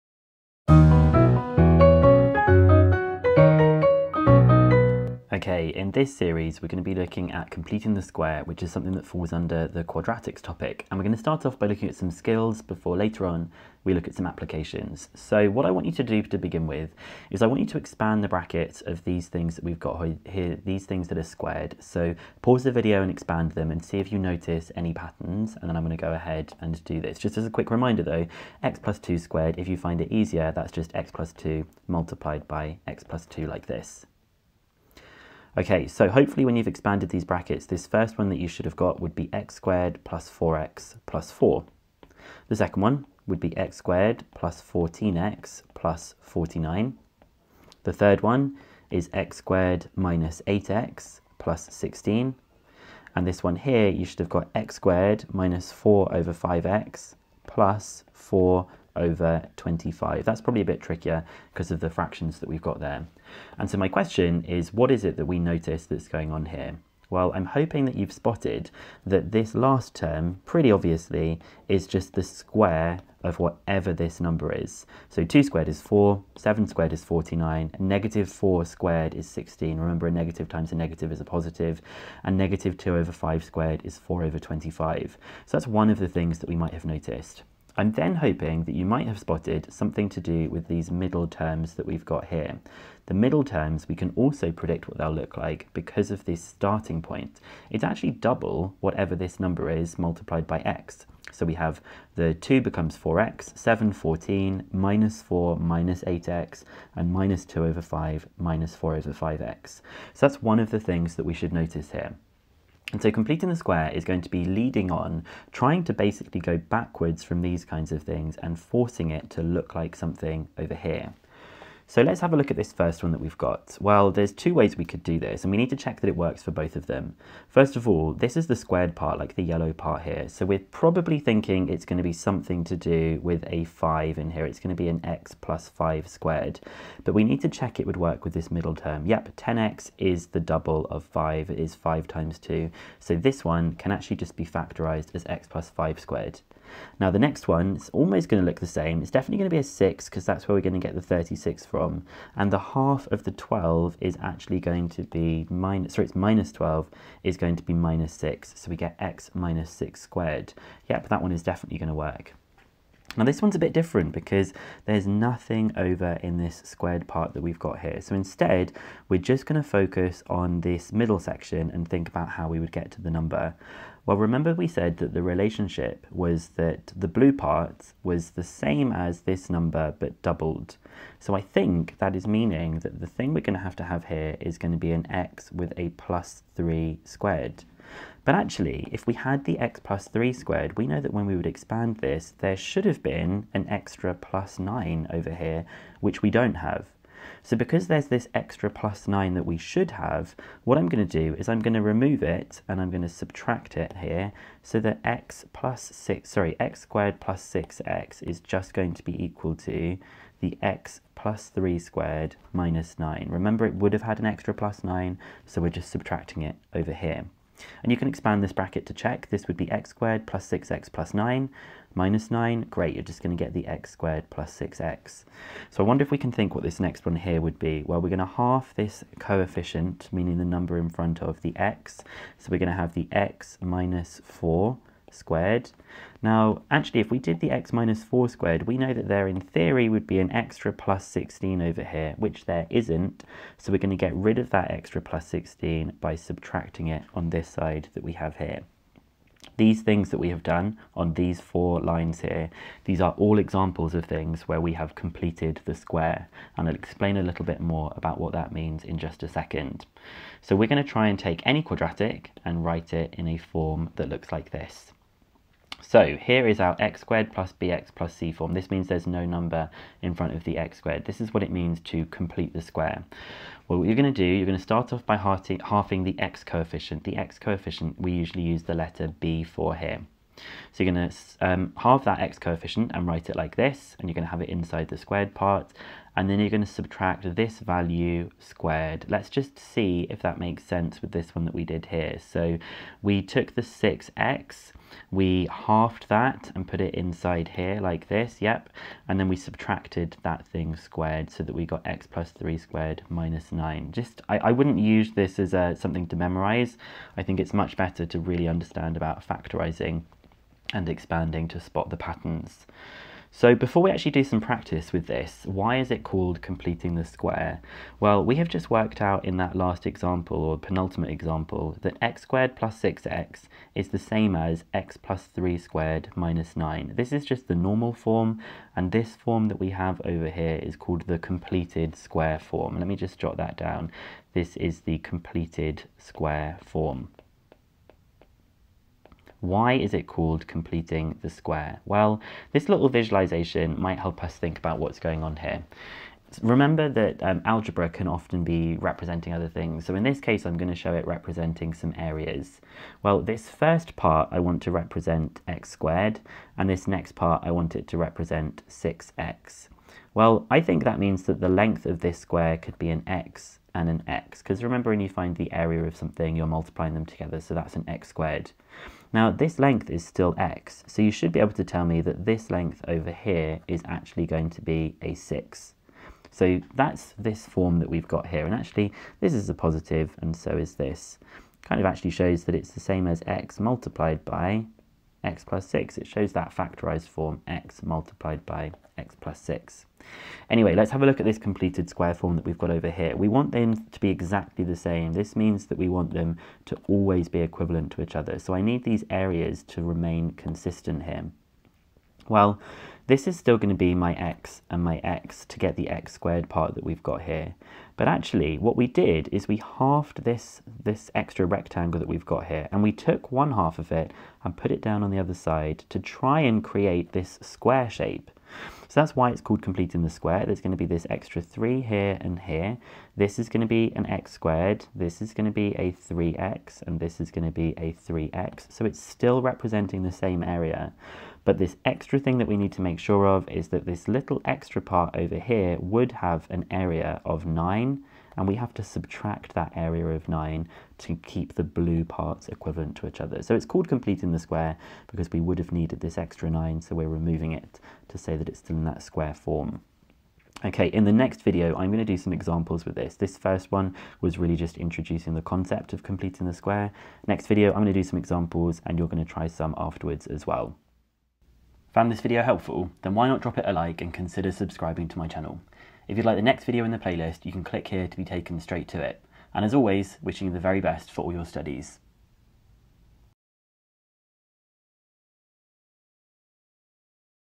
Completing the Square 1 • What is completing the square? • GCSE Maths (Higher) 🧩
These videos are designed for all exam boards, and are aimed at helping you to reach your full potential at GCSE.

0:00 Expanding squares - pattern spotting
1:53 Going backwards
4:32 Method and visual representation
7:41 Outro